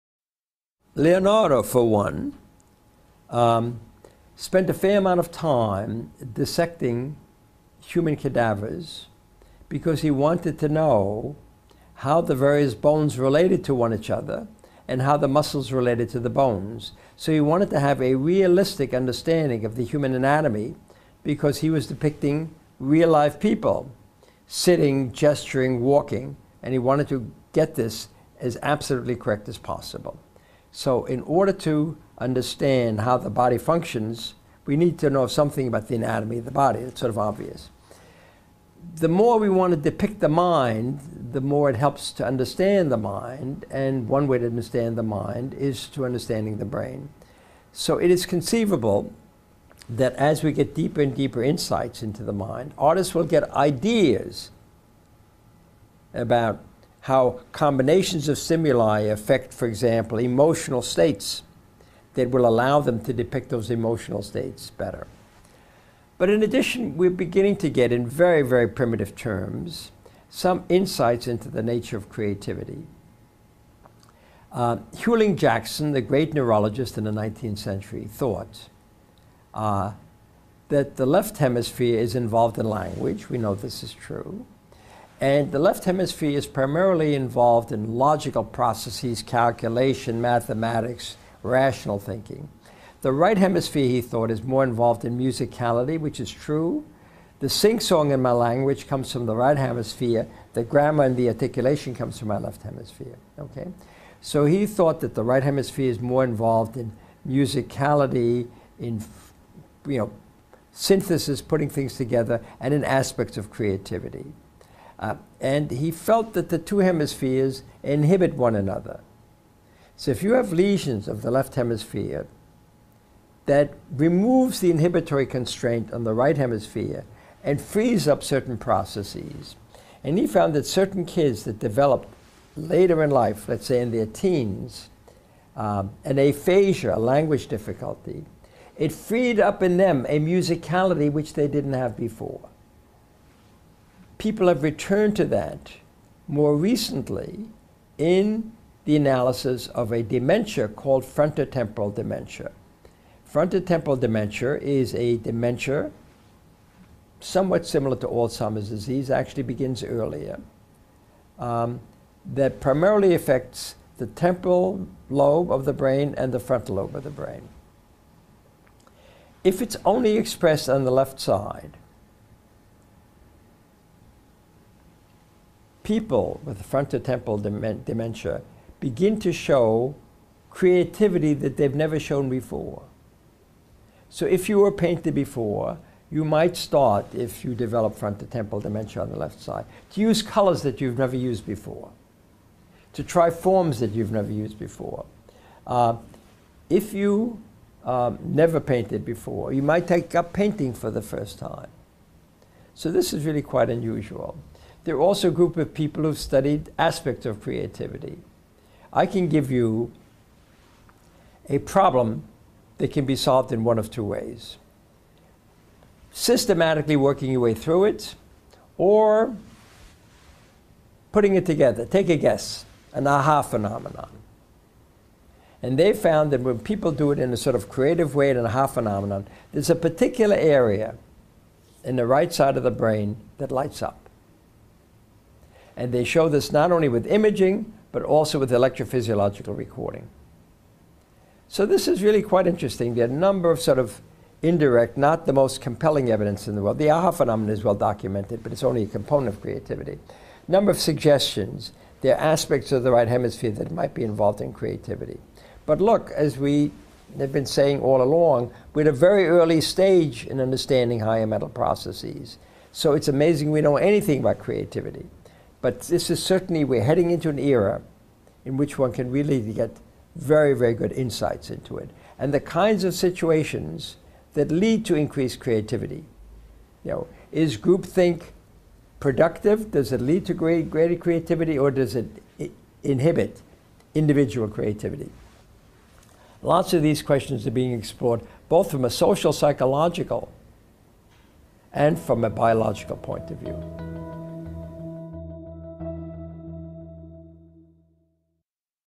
Eric Kandel- Criatividade, seu cérebro e o momento AHA! (Legendado)
Big Think com o neurocientista Eric Kandel.
Traduzido pela equipe NeuroeEduca.